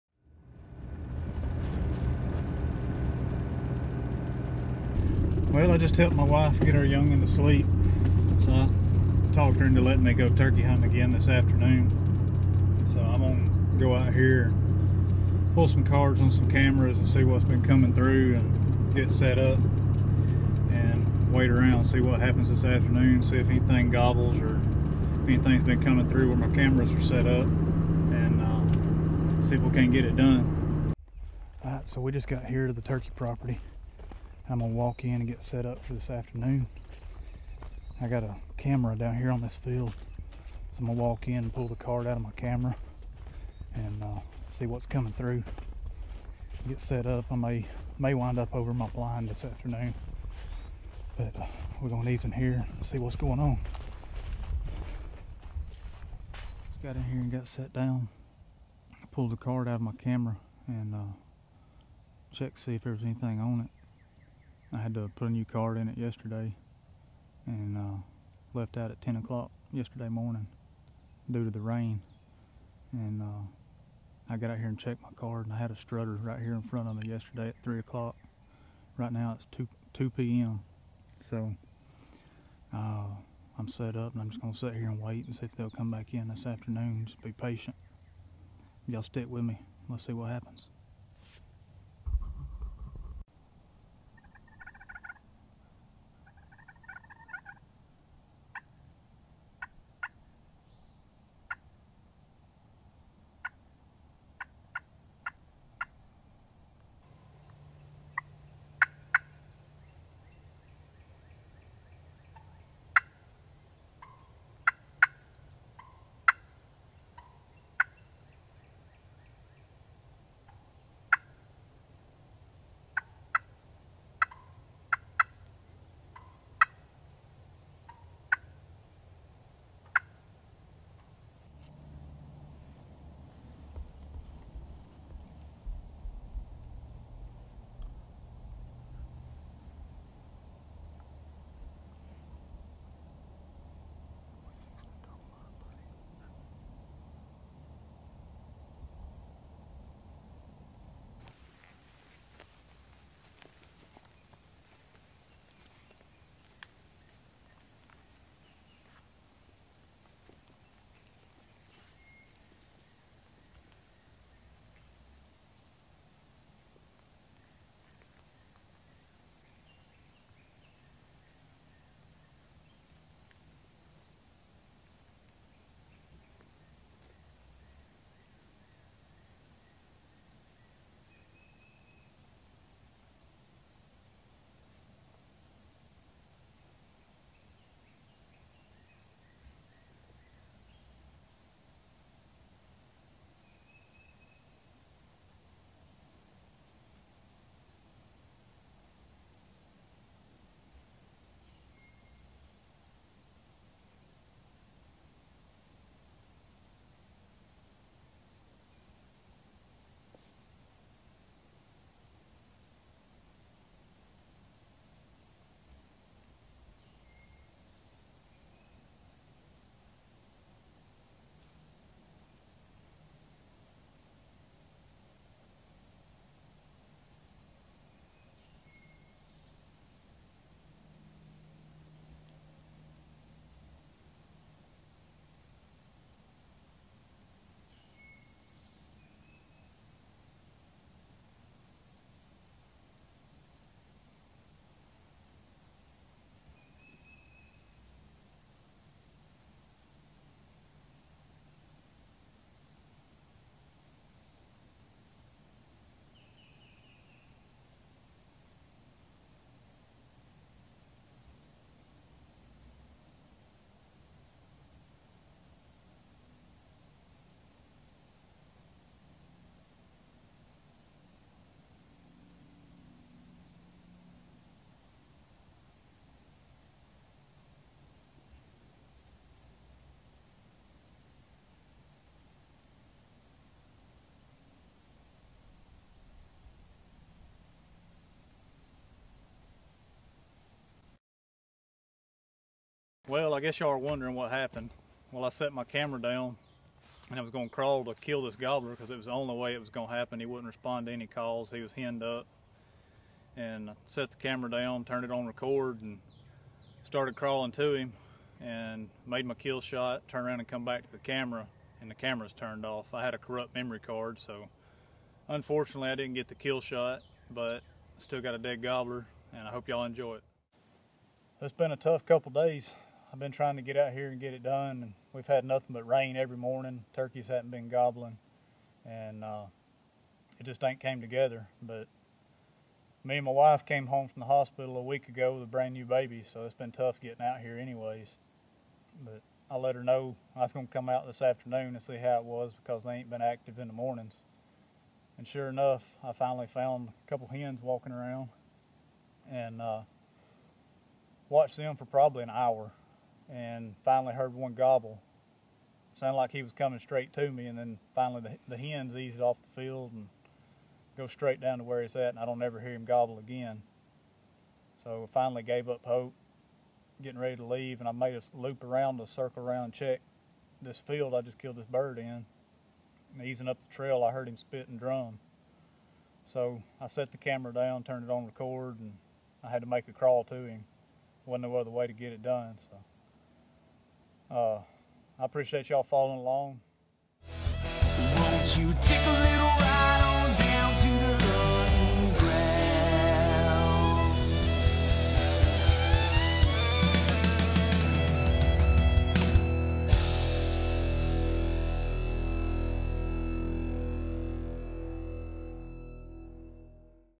GOBBLER CRAWL SUCCESS! Turkey Ambush S10
Jeremy puts the gobbler crawl on a big ol tom in Alabama and has success!